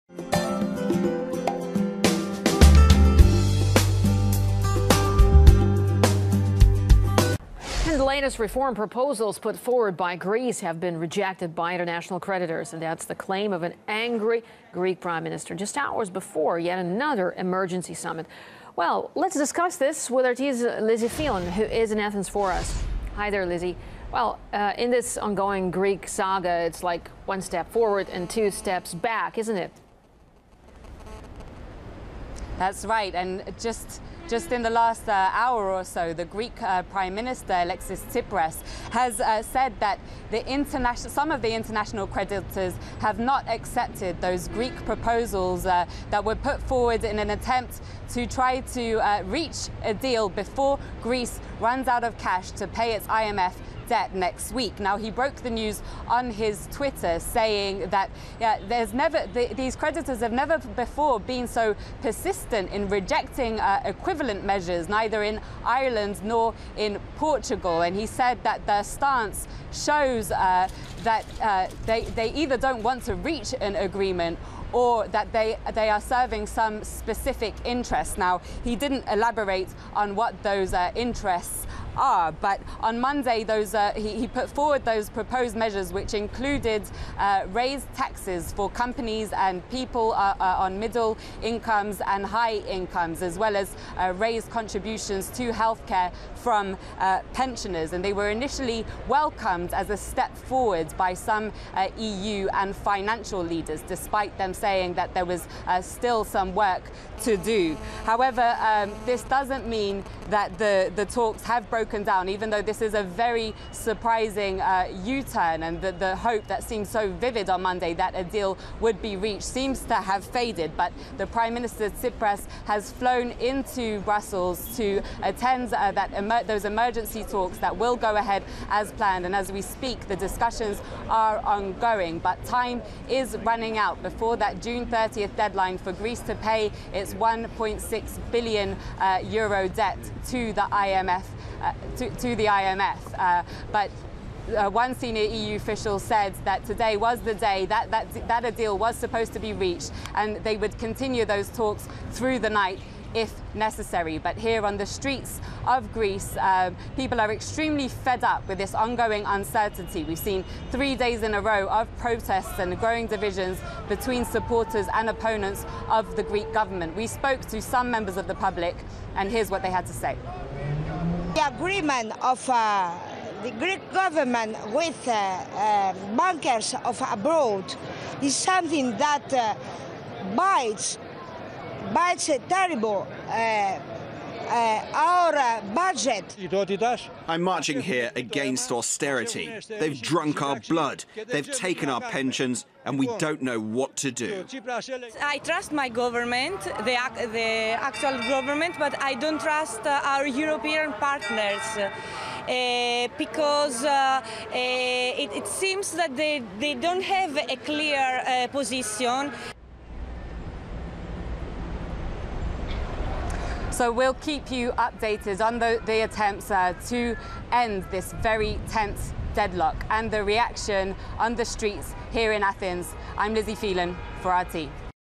EU creditors deny Greek reform plan - Now what?
Prime Minister Alexis Tsipras said the international creditors didn’t accept the new Greek proposals prior to another round of negotiations on Wednesday. Fears are escalating Greece could default and leave the eurozone.